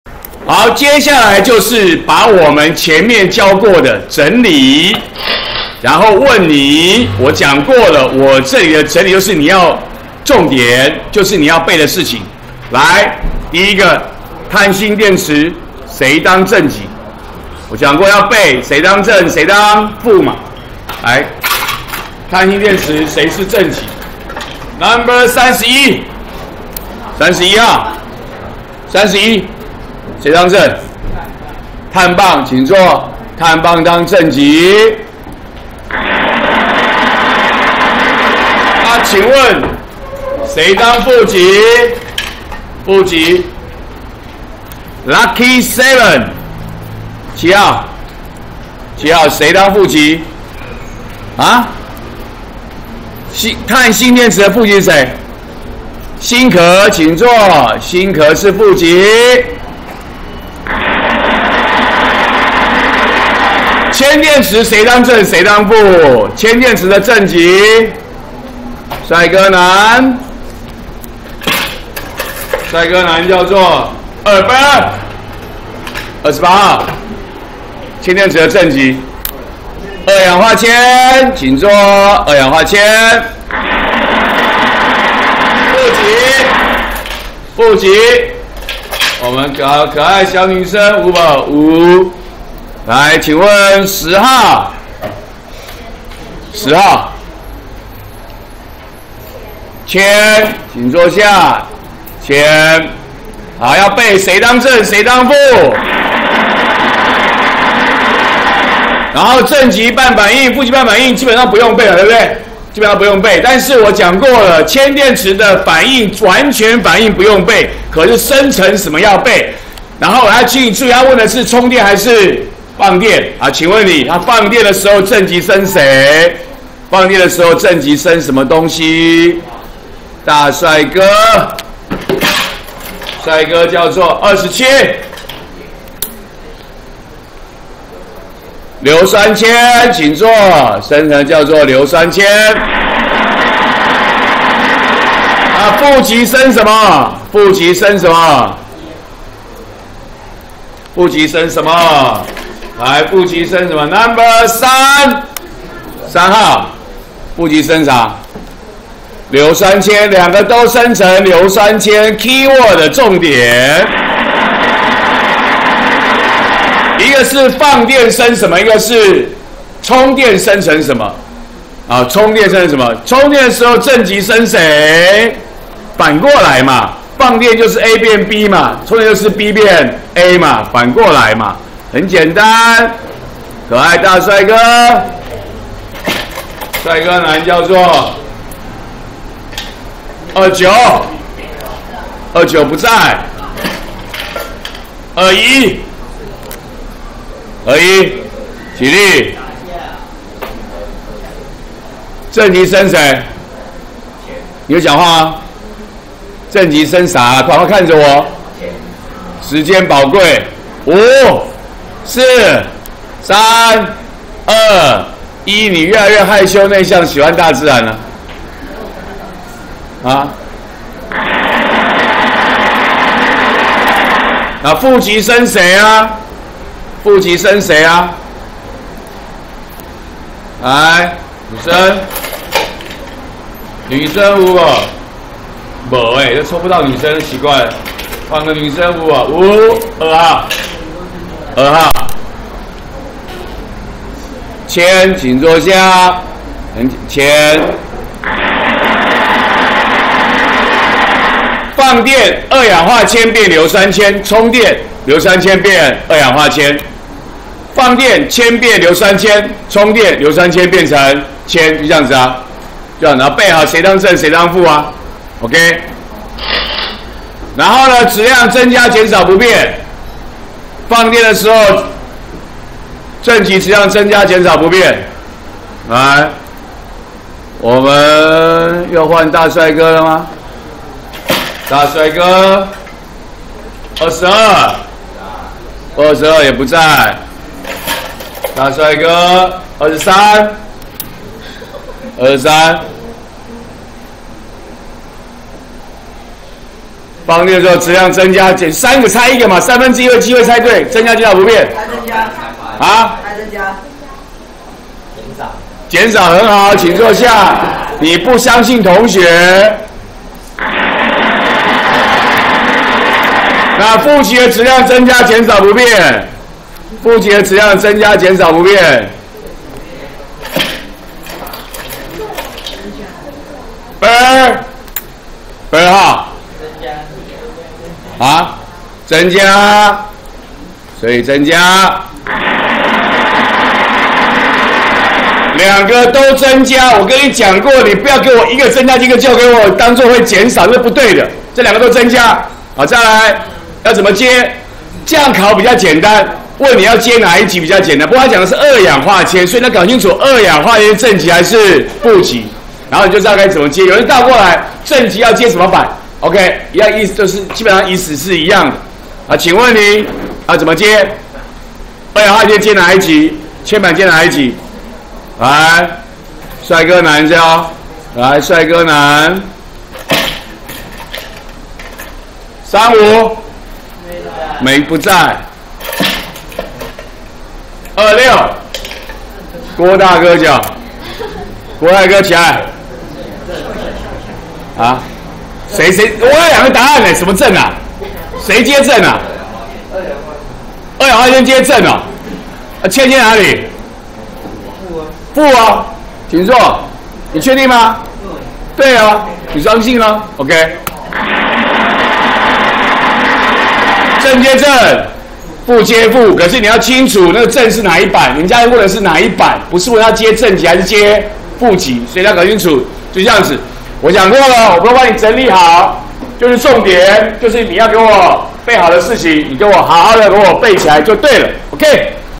104學年國三理化＿電化學12：乾電池、鉛電池整理
整理並詢問同學，關於乾電池、鉛電池應理解、記憶的事項。